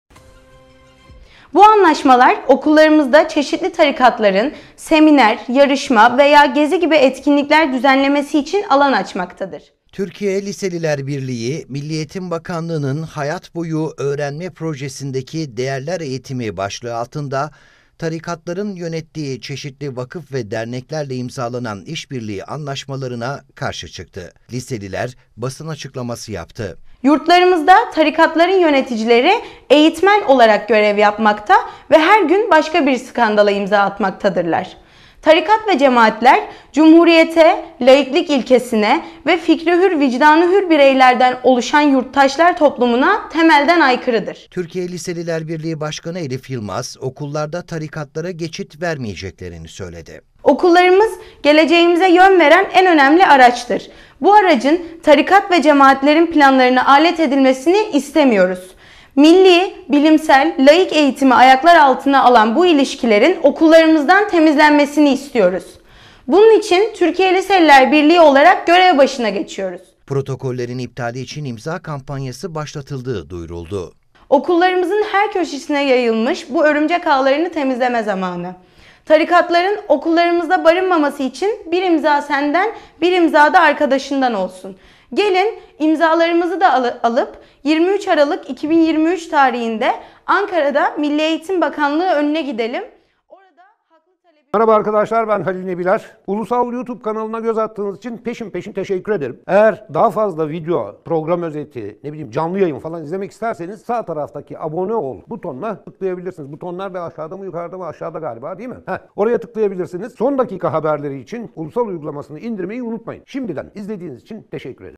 MEB tarikatlarla anlaşma imzalıyor! Liseliler: "Yırtıp atacağız!"
Milli Eğitim Bakanlığı’nın tarikatların yönettiği çeşitli vakıf ve derneklerle protokol imzalamasına Türkiye Liseliler Birliği'nden tepki geldi. Türkiye Liseliler Birliği Başkanı Elif Yılmaz, "Okullarımızda tarikat ve cemaatleri istemeyen tüm liselileri imza atmaya çağırıyoruz" dedi.

AE-1223

Türkiye ve dünya gündemindeki haberler, son dakika gelişmeleri Ulusal Kanal'da!